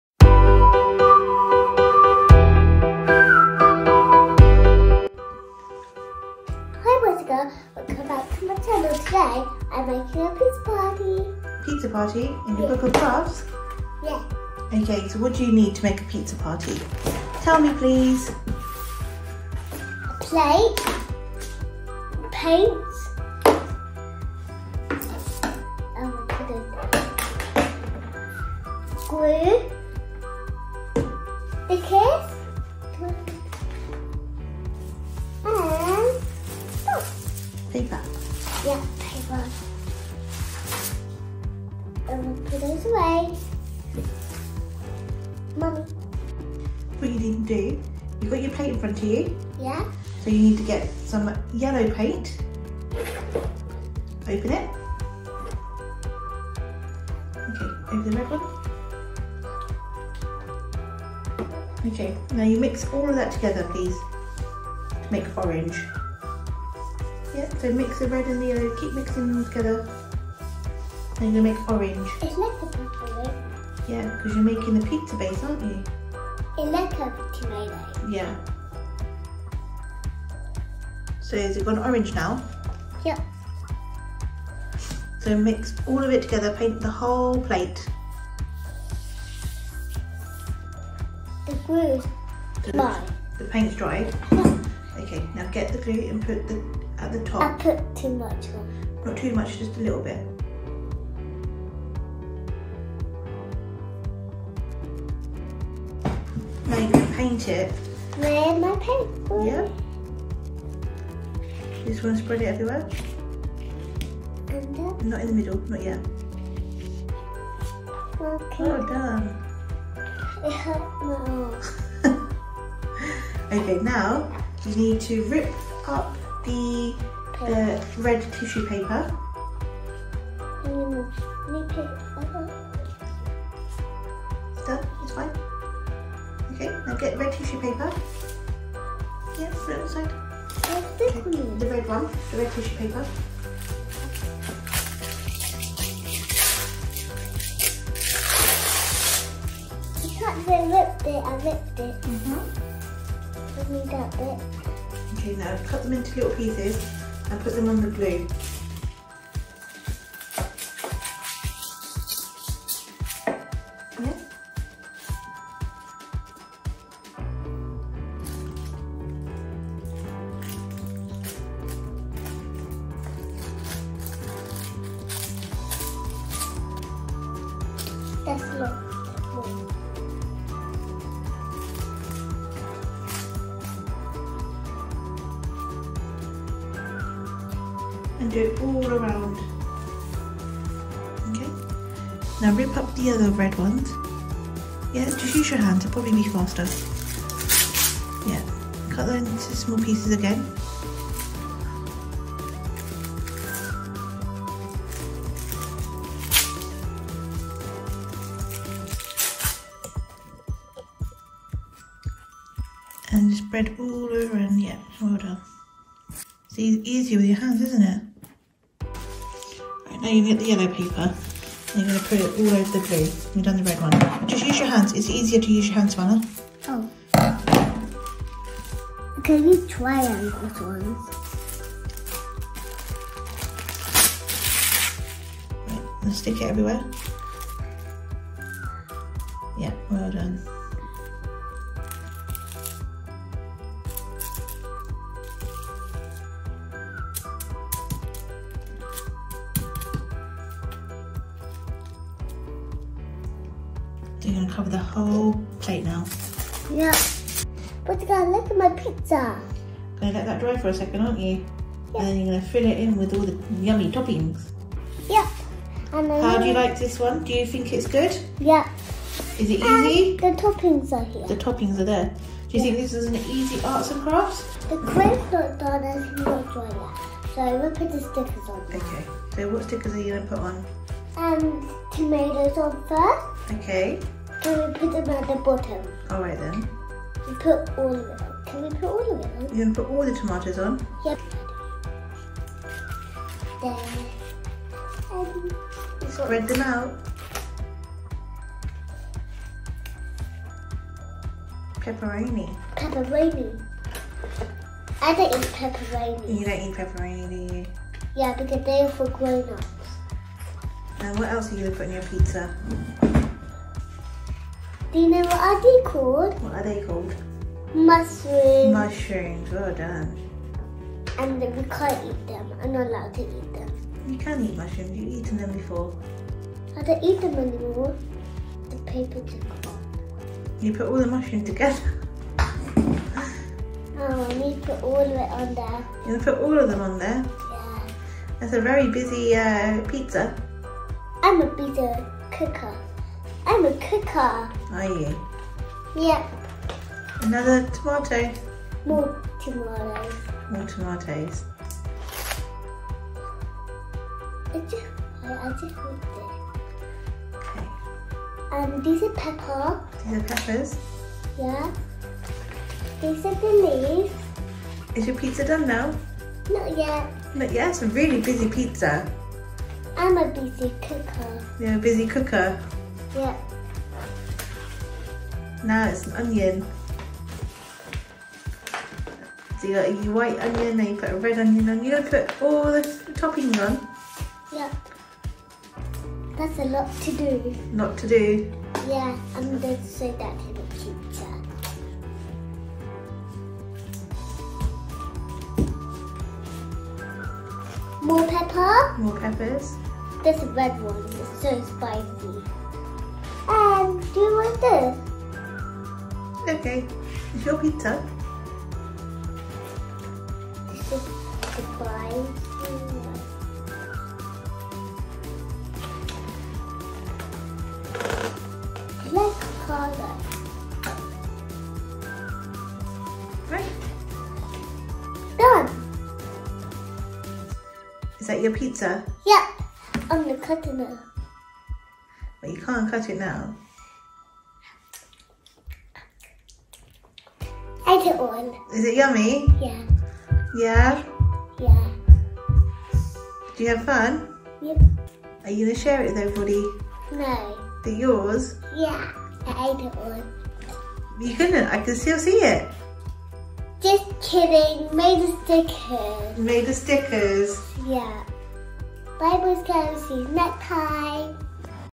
Half Term Fun!! Make a Pizza with Savannah using arts and crafts!
Its half term and I wanted to make a pizza with my arts and crafts set to show my teacher at school!!!

It was so simple to do!